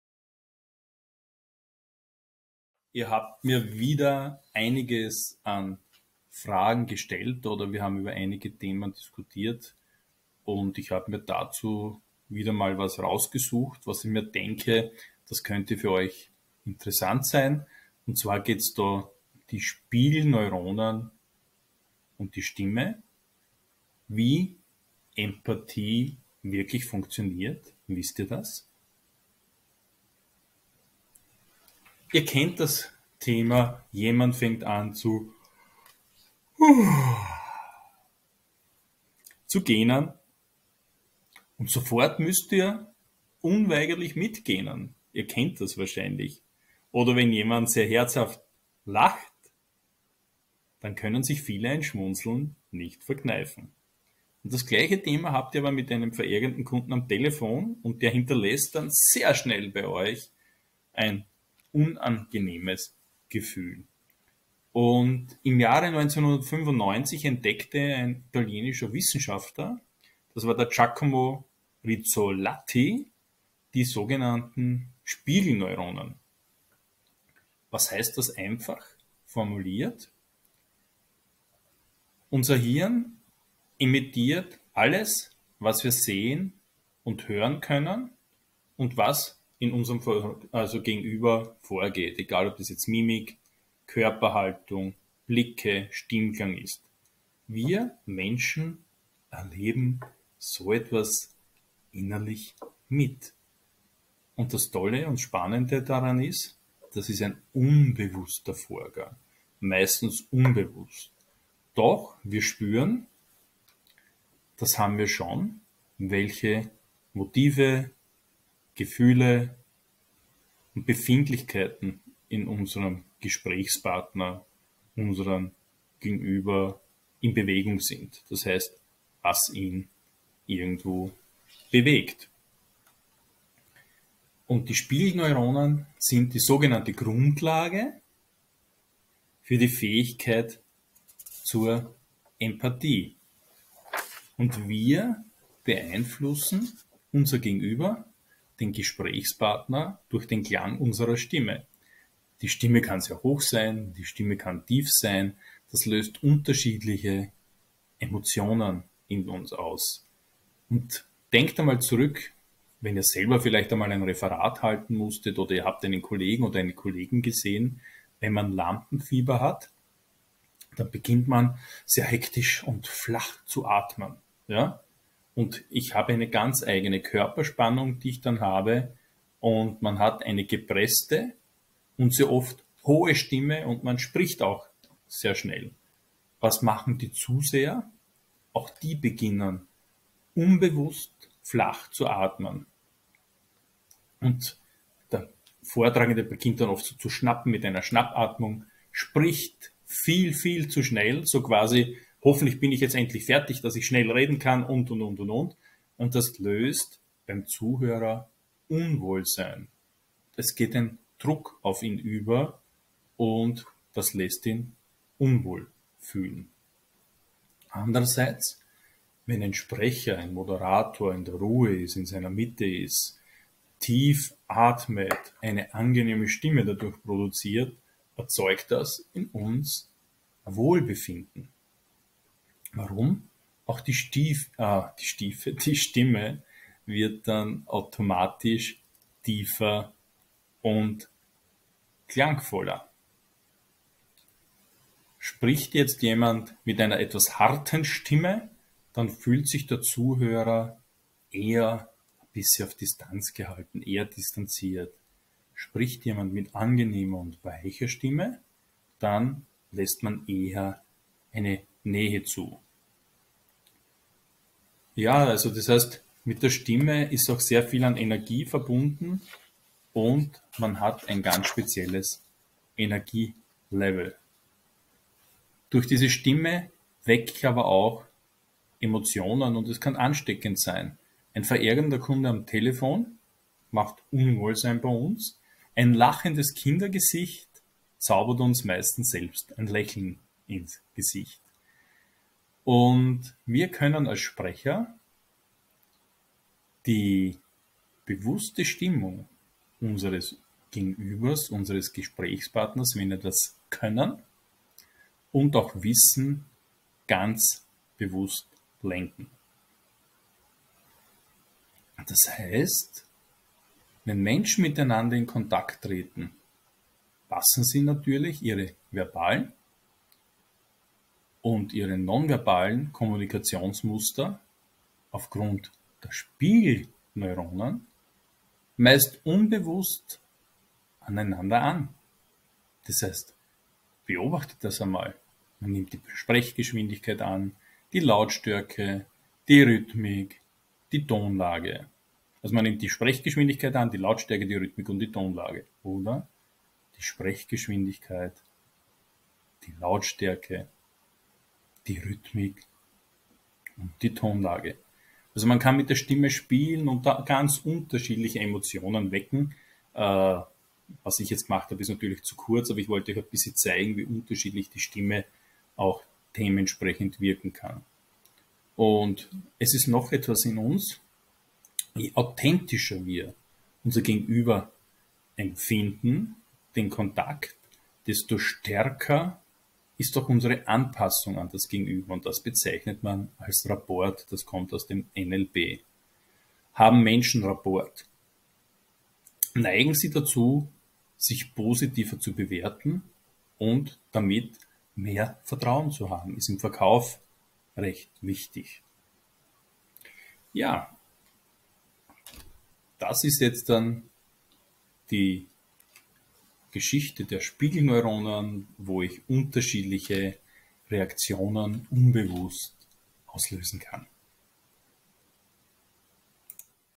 Führung & Verkaufspsychologie | Was sind Spiegelneuronen? - Stimme und Empathie
*Thema:* Was sind Spiegelneuronen? - Stimme und Empathie
*Kurs:* Führung und Verkaufspsychologie | Wissensvideo 14
*Dozent:* Raimund Hütter

📖 *Deine Vorteile*
▸ Förderungen möglich